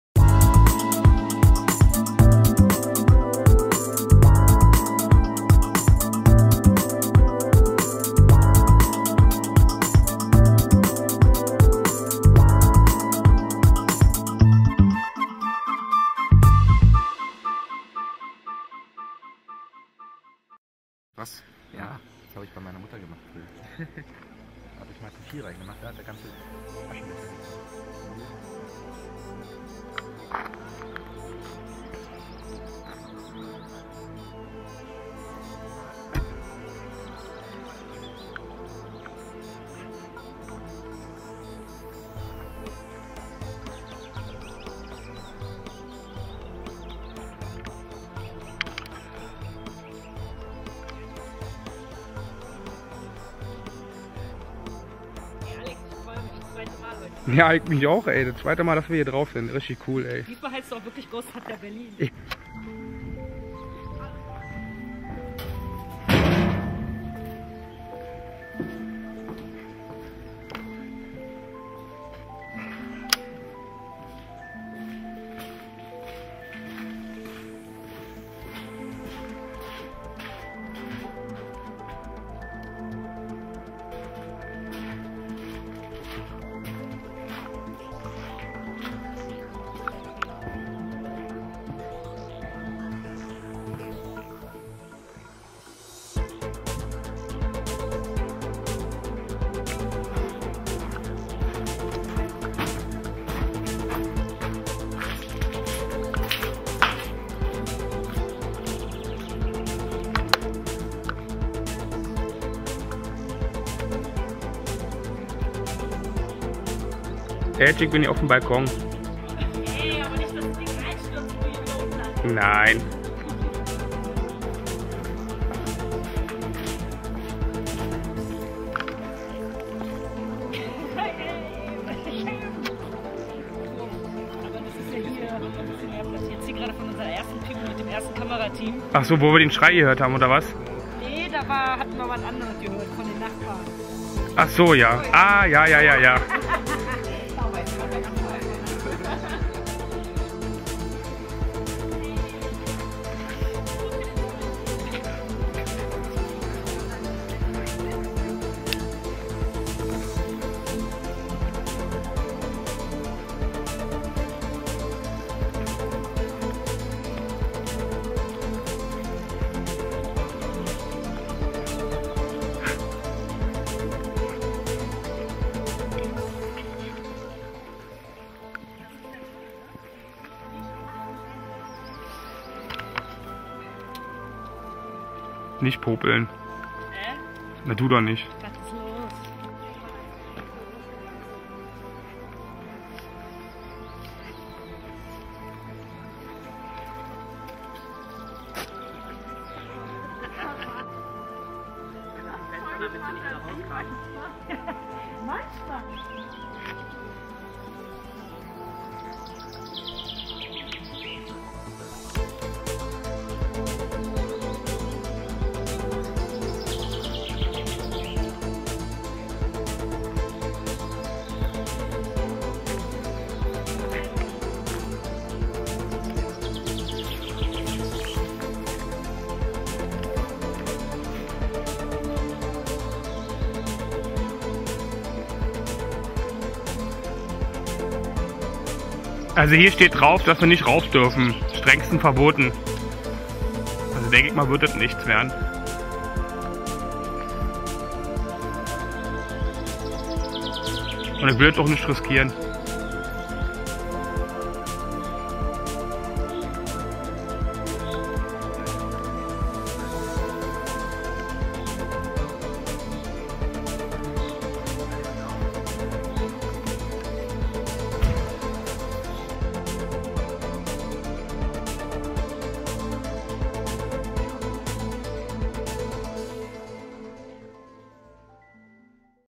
Gemeinschafts PU mit dem G.E.T. Team und Ghosthunter Berlin
Gemeinschafts PU mit dem G.E.T. Team und Ghosthunter Berlin am 21. April 2012